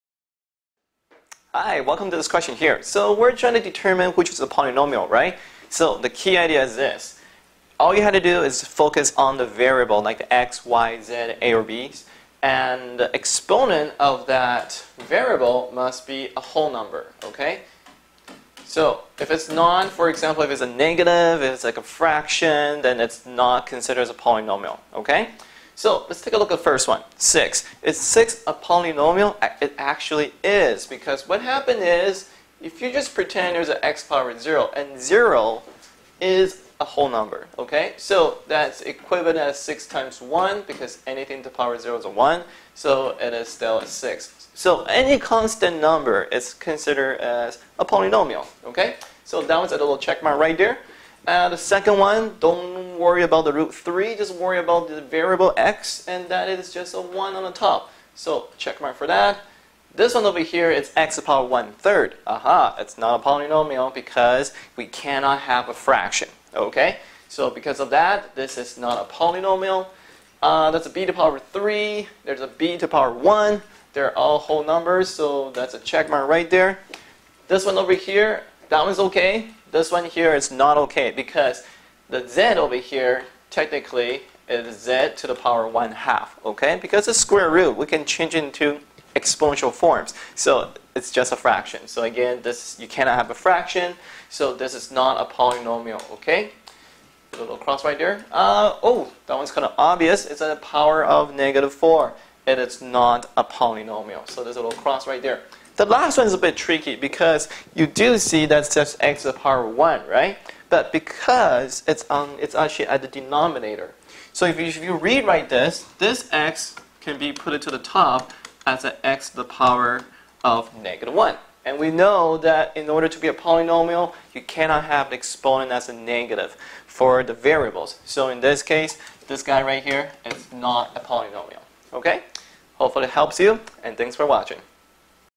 ʕ•ᴥ•ʔ What is a Polynomial Explained Properly.. Finally!
Quickly master how to determine what is a polynomial.

There are criteria for expressions to be called polynomials. A polynomial needs to have at last 3 algebraic terms. For instance, an expression with negative exponents is not a polynomial. In this lesson, we will also learn how to classify polynomials base on their number of terms.

Hi, welcome to this question here. So we're trying to determine which is the polynomial, right. So the key idea is this; all you have to do is focus on the variable like x, y, z, a or b's; and exponent of that variable must be a whole number, okay. So, if it's known, for example it's a negative, it's like a fraction then it's not considered as a polynomial, okay. So let's take a look at the first one, 6. Is 6 a polynomial? It actually is because what happened is, if you just pretend there's a X power zero; and zero is a whole number, okay. So, that's equivalent as 6 x 1 because anything to the power zero is a 1, so it is still 6. So any constant number is considered as a polynomial, okay. So, it will check mark right there. The second one, don't worry about the root 3 just worry about the variable X and that is just a 1 on the top, so, check mine for that. This one over here is X to power 1/3. A-ha! It's not a polynomial because we cannot have a fraction, okay. So because of that this is not a polynomial. Aah, there's "b" b to power 3, there's a "b" to power 1; they're all whole numbers so that's a check mark right there. This one over here, that one is okay. This one here it's not okay because the Z over here, technically, it's a Z to the power one half, okay, because it's a square root; we can change it into exponential forms. So it's just a fraction, so again this, you cannot have a fraction so this is not a polynomial, okay. We'll cross right there. That one is kinda obvious, it's a power of -4 and it's not a polynomial, so this will cross right there. The last one is a bit tricky because you do see that's X to the power 1, right. But because it's actually at the denominator. So if you rewrite this, this X can be put to the top, that's the X to the power of -1 and we know that in order to be a polynomial, you cannot have exponent as a negative for the variables. So in this case, this guy right here is not a polynomial, okay.